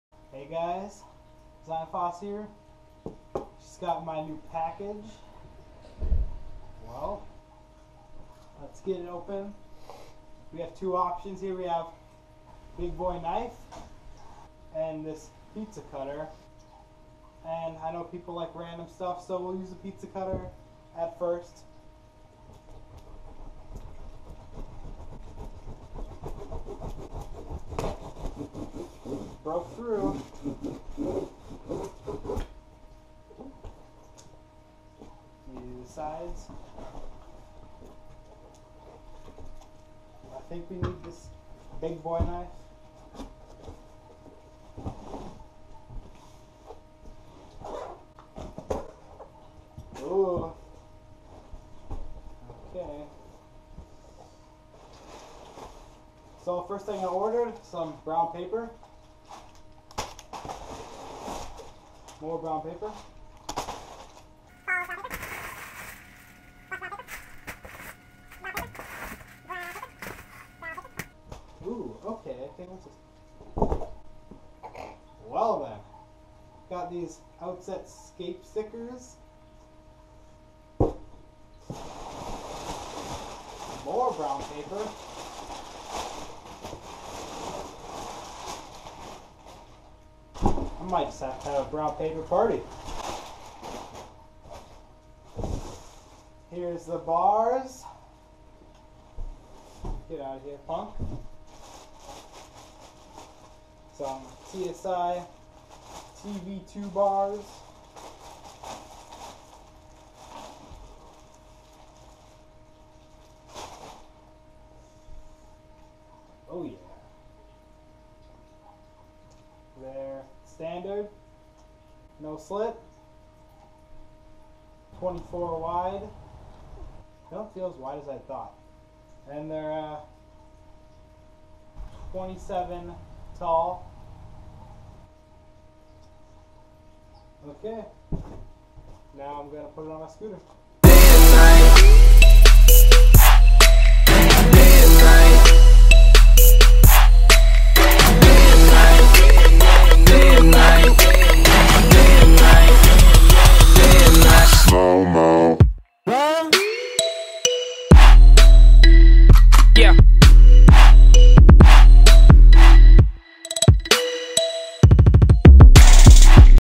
TSI TV 2 Bar Unboxing | OUTSET SELECT
I received my package today so I filmed an unboxing.

Filmed & Edited By Zyan Foss